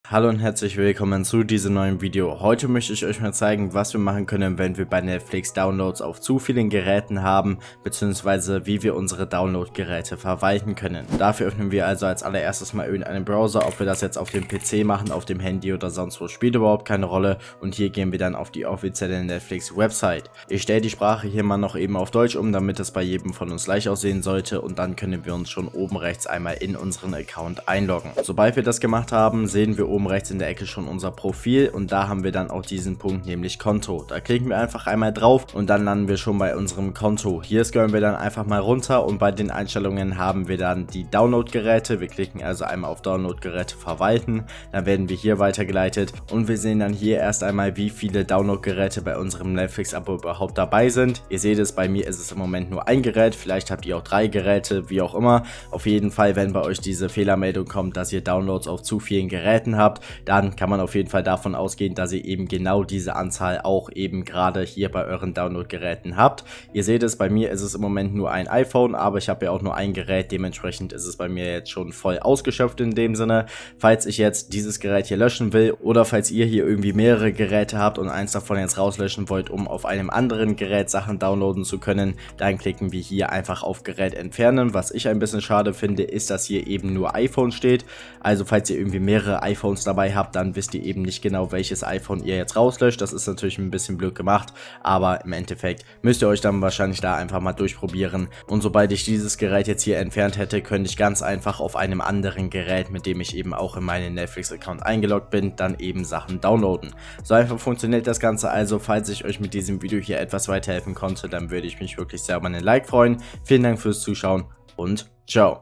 Netflix Downloads auf zu vielen Geräten Fehler beheben | Download-Geräte verwalten
In diesem Video zeige ich euch, was ihr machen könnt, wenn ihr bei Netflix die Fehlermeldung bekommt, dass ihr Downloads auf zu vielen Geräten habt, bzw. wie ihr eure Download Geräte verwalten könnt.

Korrekt wenn ihr ein Abo und Like dalasst, Kuss